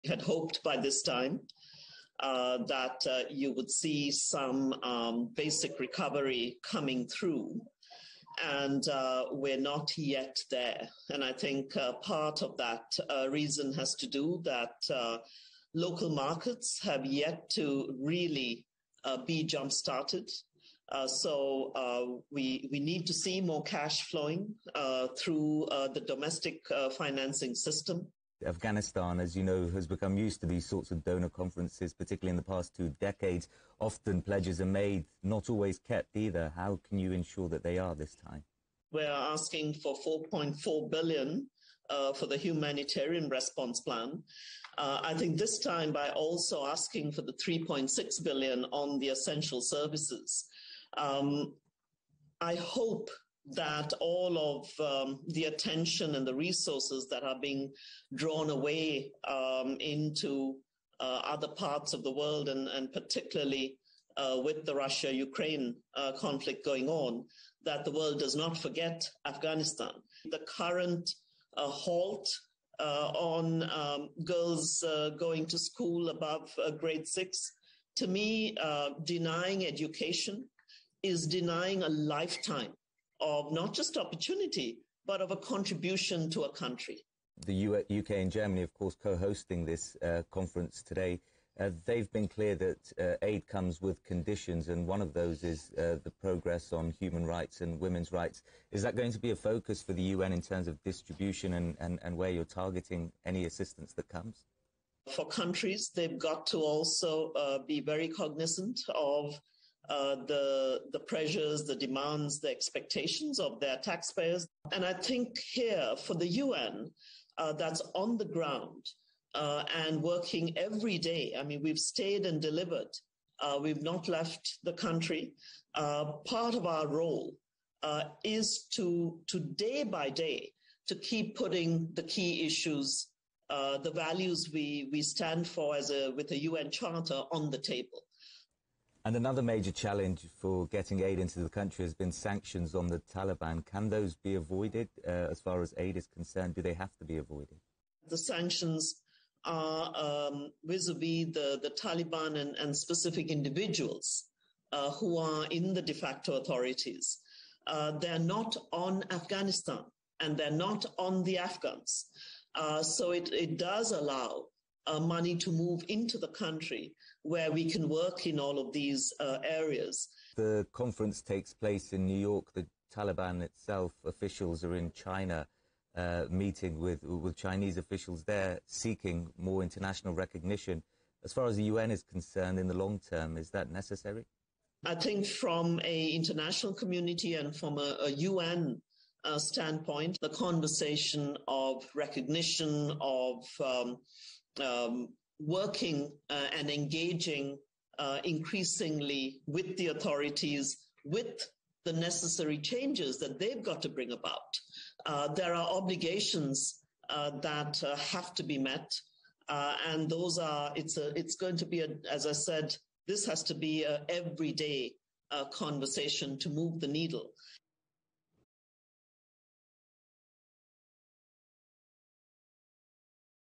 On CGTN at 2022/3/31 16:50
As the conflict in Ukraine grabs the headlines, there is a different crisis taking place in Afghanistan. The UN is trying to raise funds to avert the worse scenario.